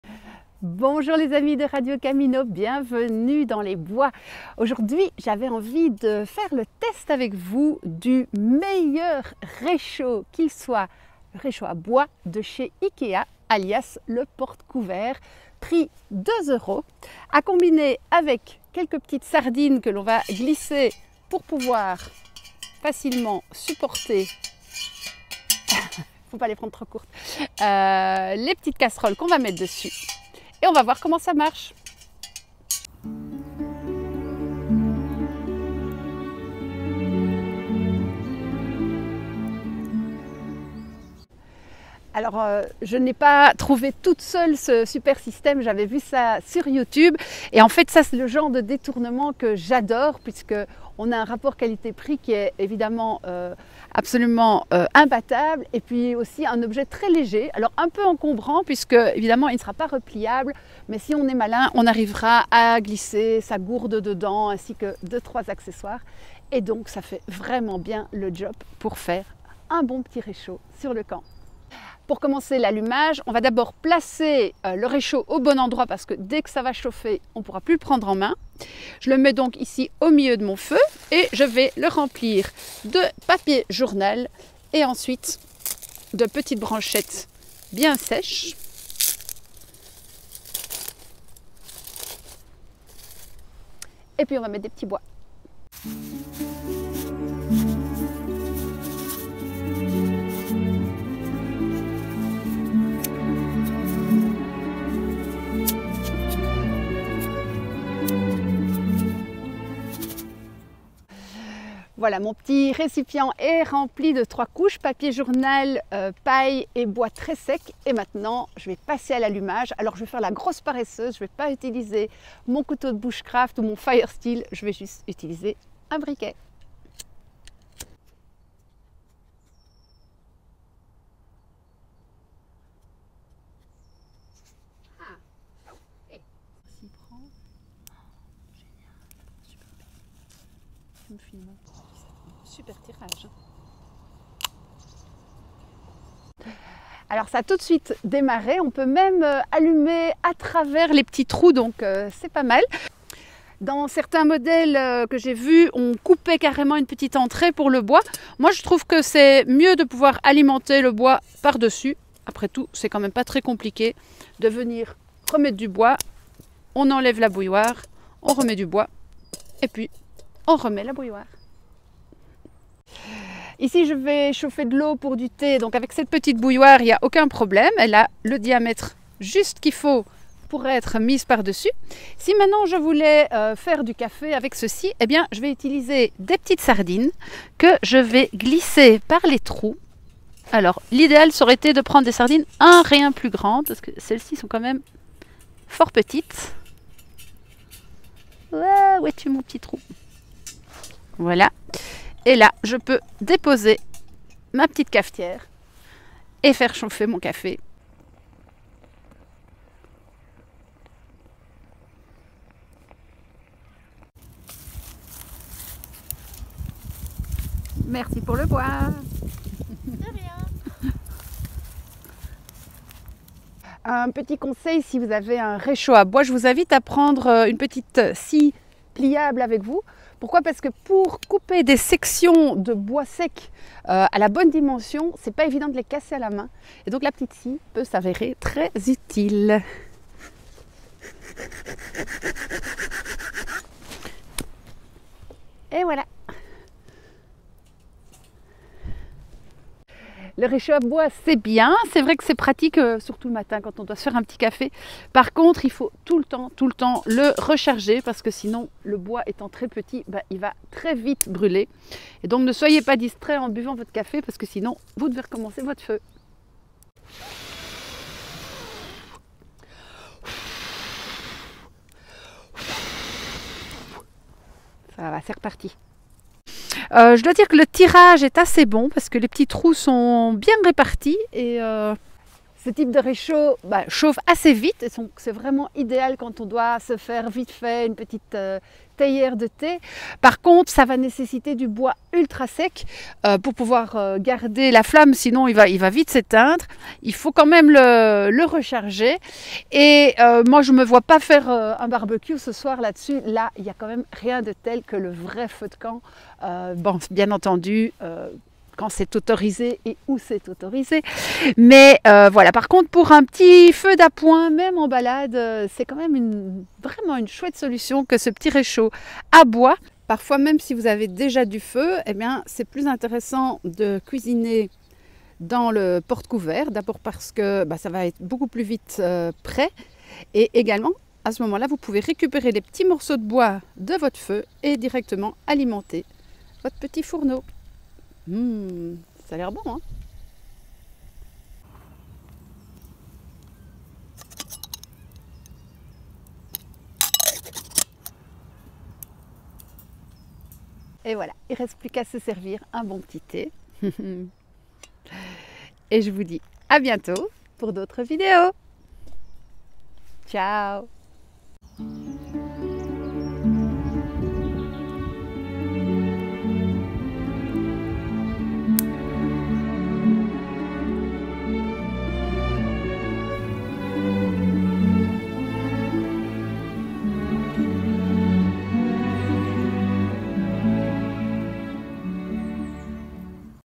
Le réchaud à bois (égouttoir à couverts) IKEA
Un porte-couverts détourné en réchaud à bois, prix 2 euros, ultra-léger et vraiment efficace. Que rêver de mieux ? Je l'ai testé pour vous :-)